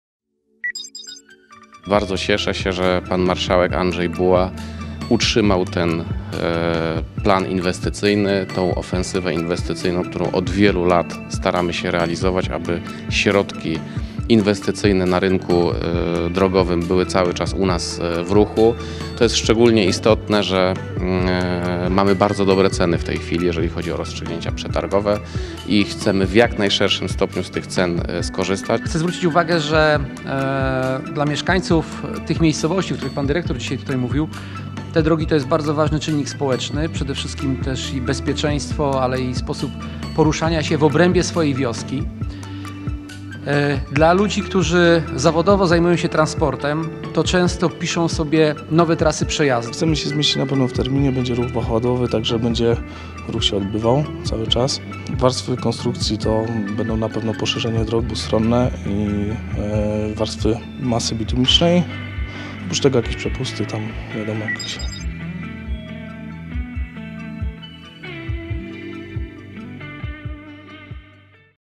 Będzie przebudowa na drodze Wojsławice- Uszyce
Droga wojewódzka nr 487 łączy powiat kluczborski z powiatem oleskim i jest najkrótszą trasą łączącą Byczynę, Gorzów Śląski i Olesno. Stanowi alternatywę dla ruchu na drogach krajowych DK45 i DK11. To już kolejny odcinek tej drogi, który będzie przebudowany. Już zima kierowcy pojada po nowej drodze 
A więcej przeczytacie tutaj